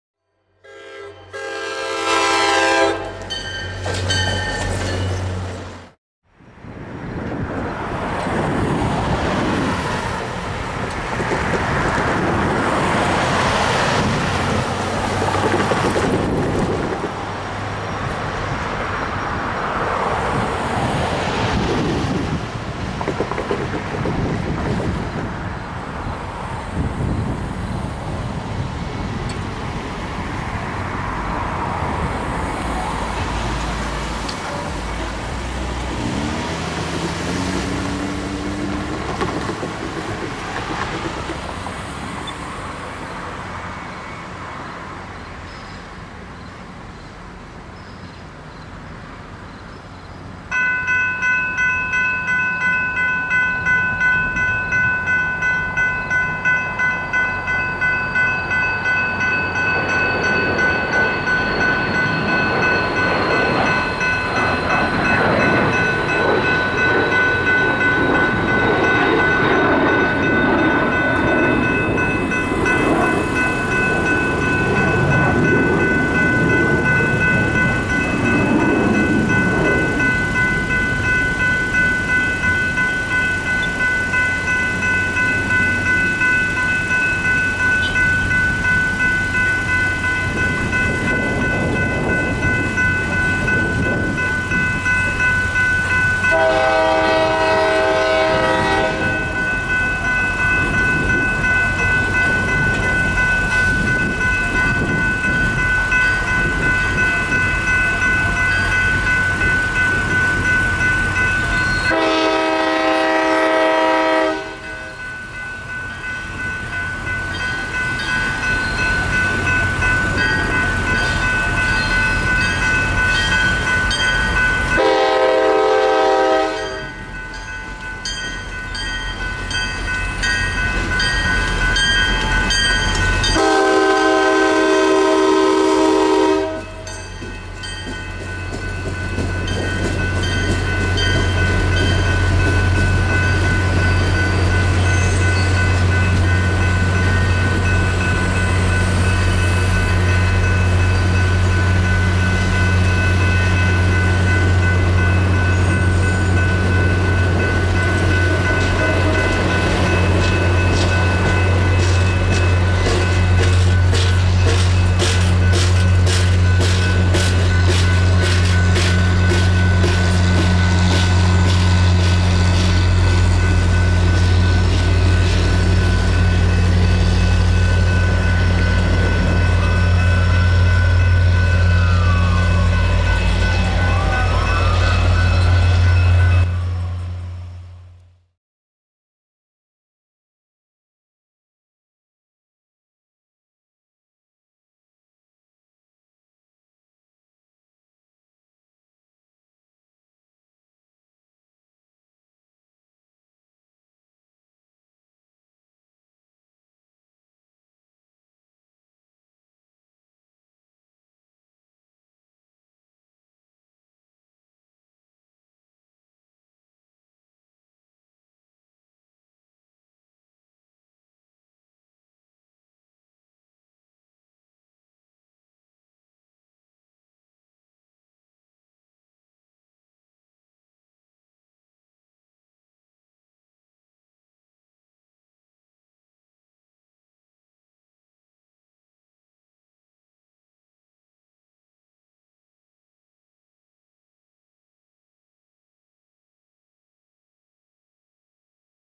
[HD]Last Capeflyer Trip Of The Year In Hyannis 9/6/21
Sadly the capeflyer is done for the year:( On the other hand, this crossing and yard is impressive from the expansion done a while ago along with the rail line signal :) I haven't been here in a long time. Enjoy:)